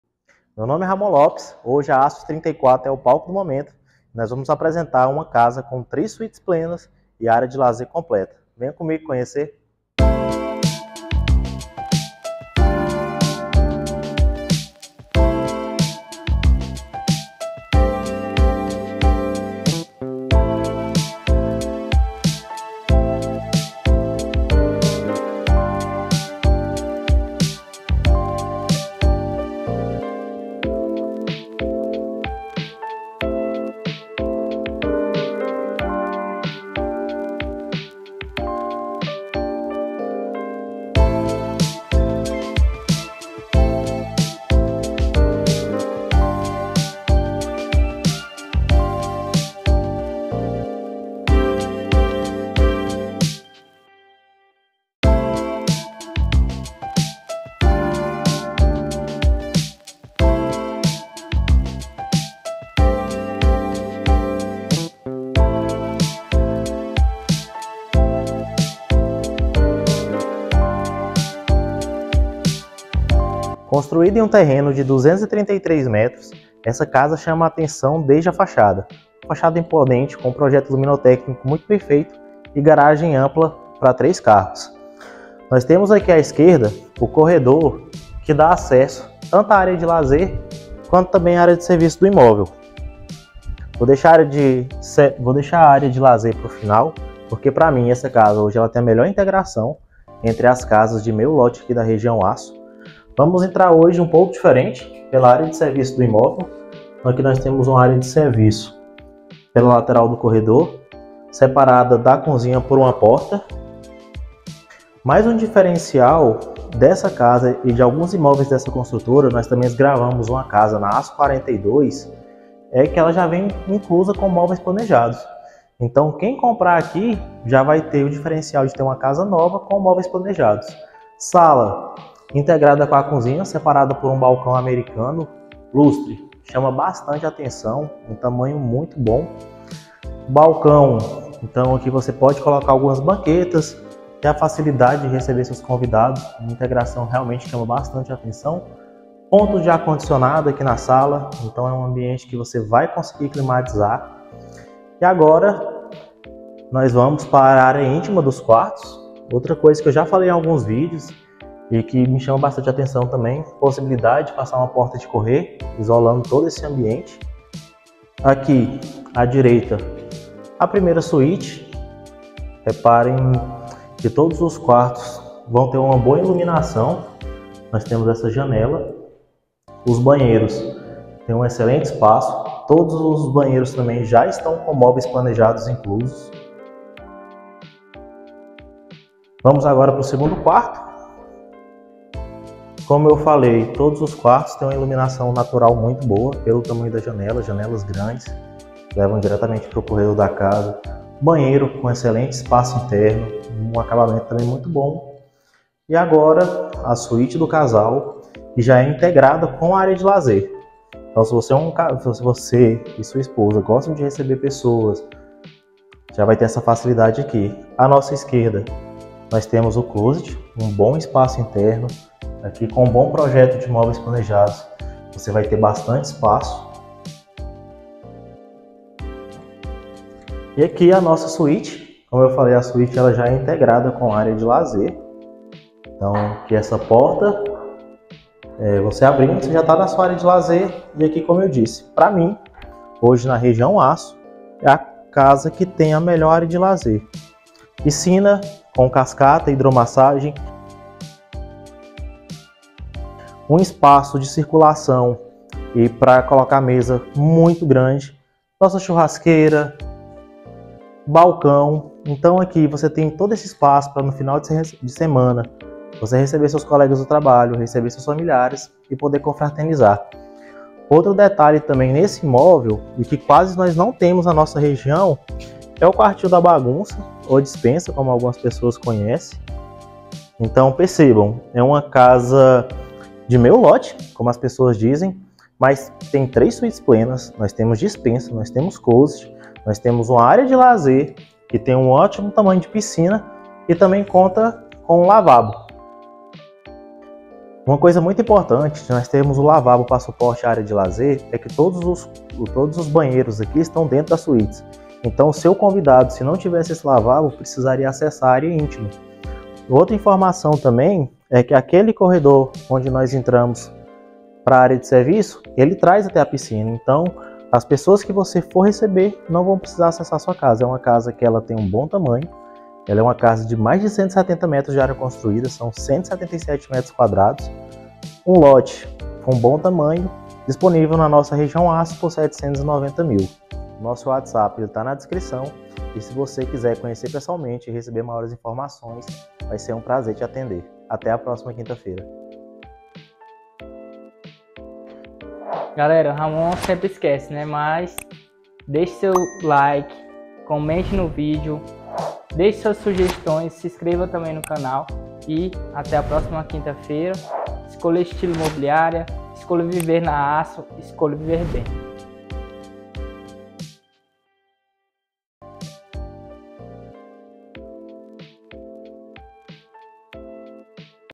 Casa com 3 Suítes plenas e área de lazer incrível na Arso 34 em Palmas-To.
O palco do momento hoje é a ARSO 34  em PALMAS-TO
Casa na quadra 309 Sul (Arso 34) com 173,22 m² de área privativa e 233,88 m² de lote em uma excelente localização.
Fachada imponente dando um charme já no início do imóvel, a garagem comporta até três carros cobertos, sala ampla e detalhes em led dando um requinte ao ambiente.
A parte íntima deste imóvel é composta por 3 quartos sendo três suítes plenas, a melhor parte da casa fica na parte dos fundos do imóvel, uma área de lazer que é completa com piscina e churrasqueira e um lavabo.
VALOR DE VENDA: R$ 790.000